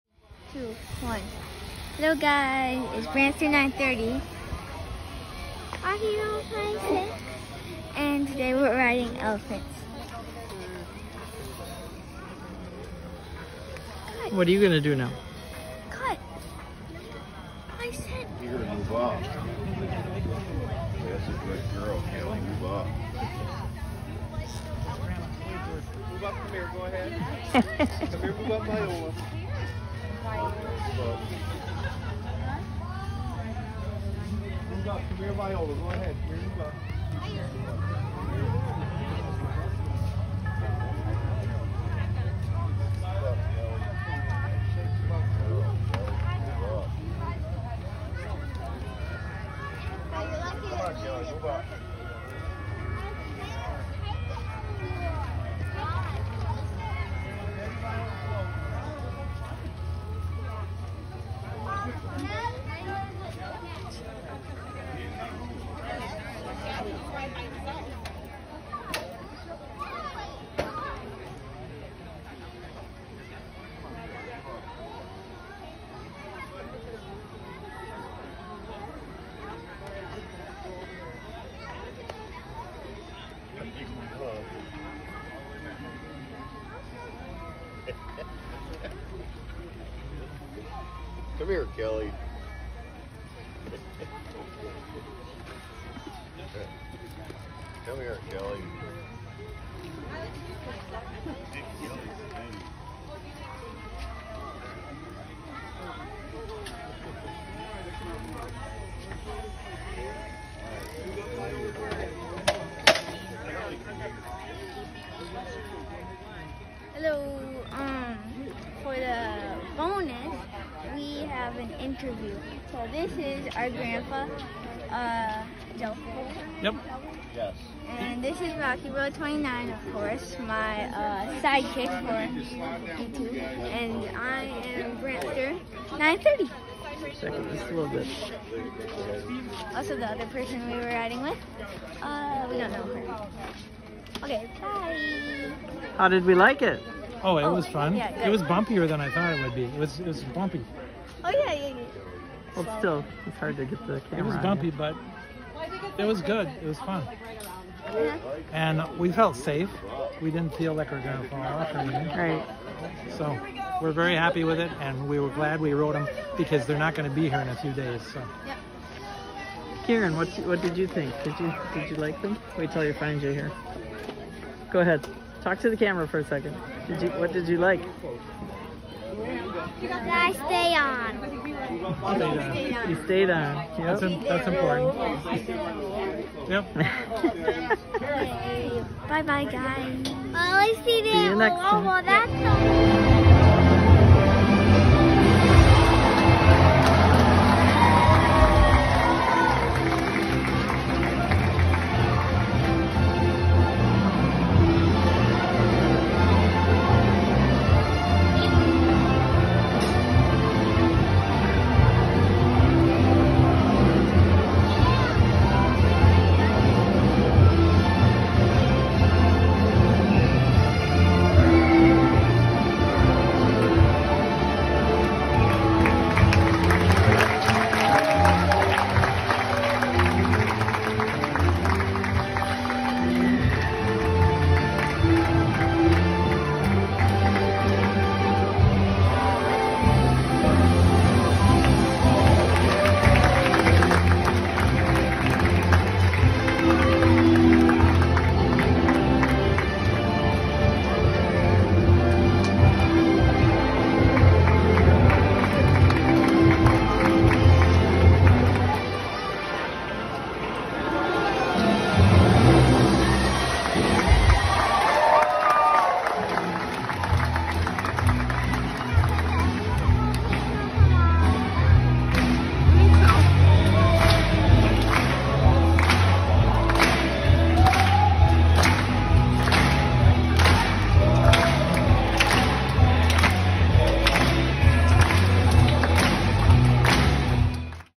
Elephant rides are fun!!!!!
I ride an elephant. in Wisconsin, circus world. they no longer have elephants, sadly.